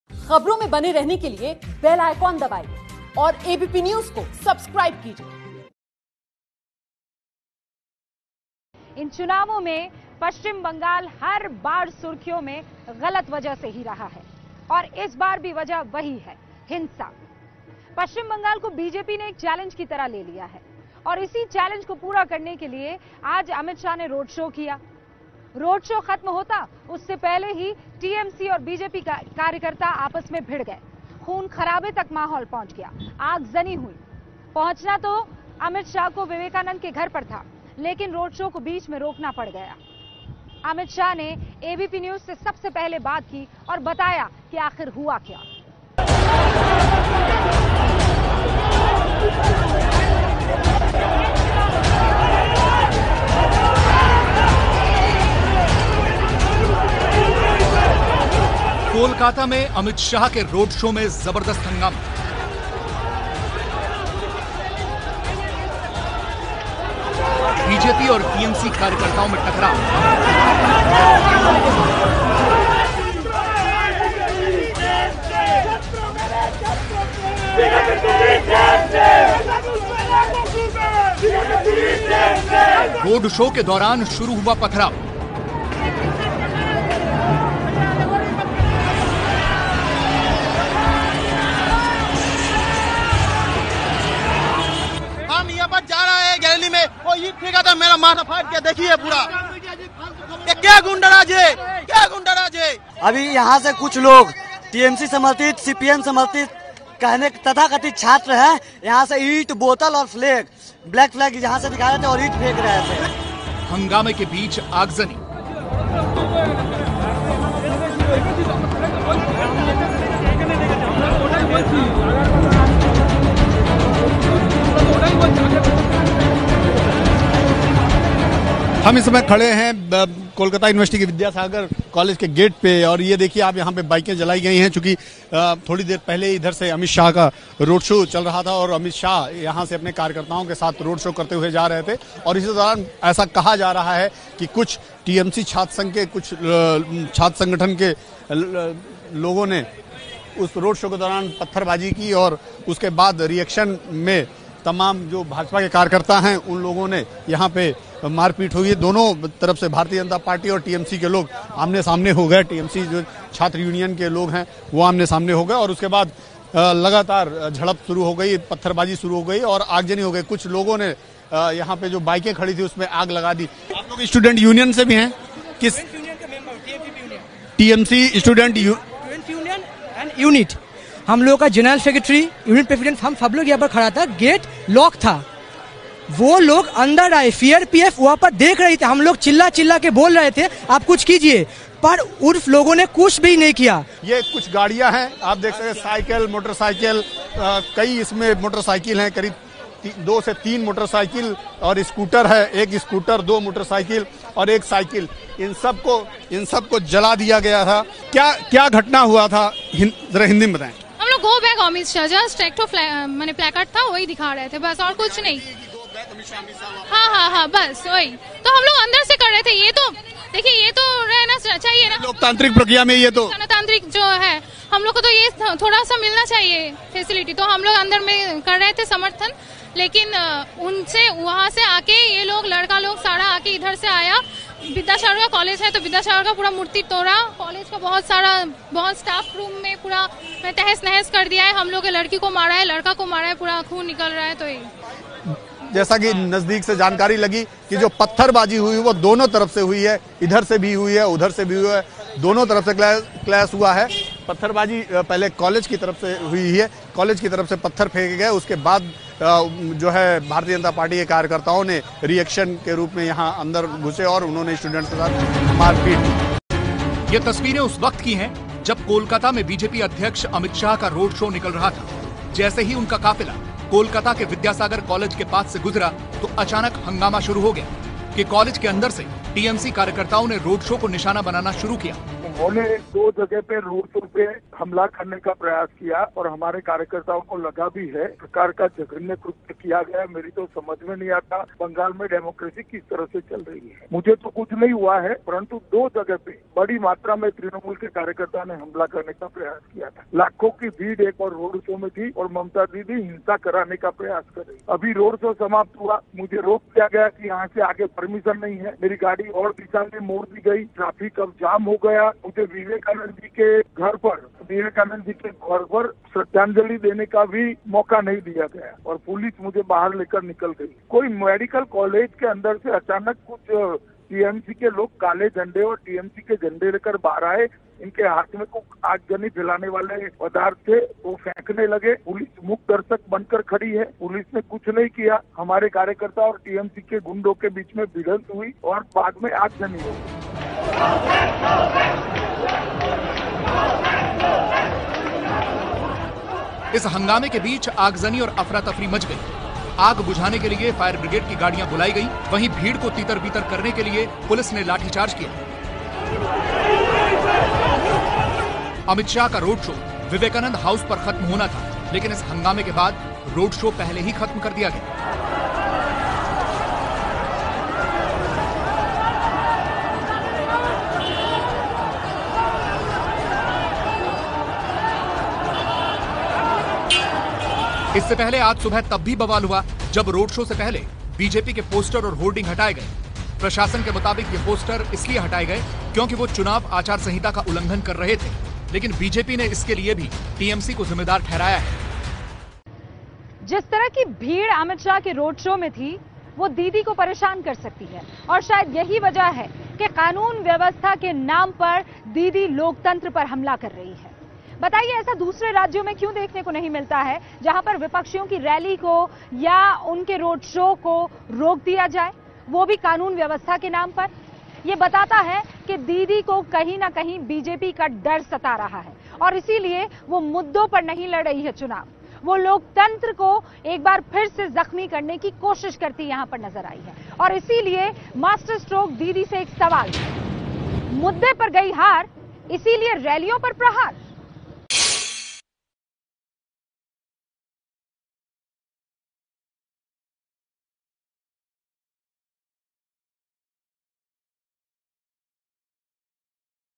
पश्चिम बंगाल में चुनावी हिंसा चरम पर, अमित शाह के रोडशो में हुई आगजनी, TMC समर्थकों पर लगा आरोप
पश्चिम बंगाल को बीजेपी ने एक चैलेंज की तरह ले लिया है और उसी चैलेंज में आज हिंसा हो गई. अमित शाह ने कोलकाता में रोड शो किया लेकिन रोड शो को जहां खत्म होना था, उससे पहले ही तृणमूल और बीजेपी कार्यकर्ताओं के बीच झड़प हो गई. बात आगजनी तक बढ़ गई. अमित शाह को रोड शो खत्म करना पड़ा. उन्हें विवेकानंद के घर जाना था लेकिन वो जा नहीं सके. हंगामे के बाद अमित शाह से सबसे पहले ABP न्यूज से बात की और बताया कि रोड शो के दौरान क्या हुआ.